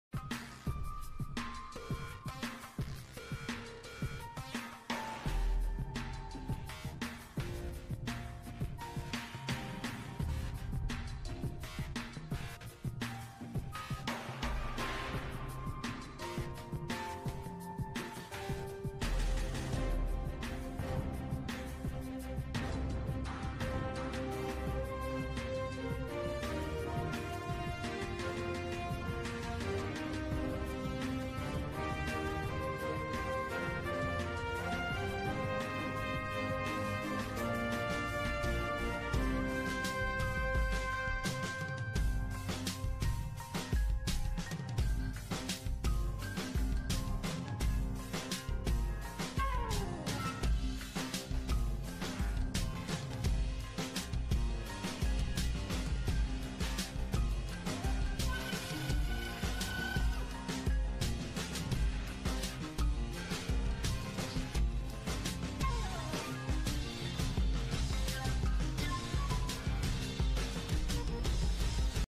Chile Vamos se cuadra con el Rechazo a la Nueva Constitución [UDI - RN - EVÓPOLI]
Nueva Constitución.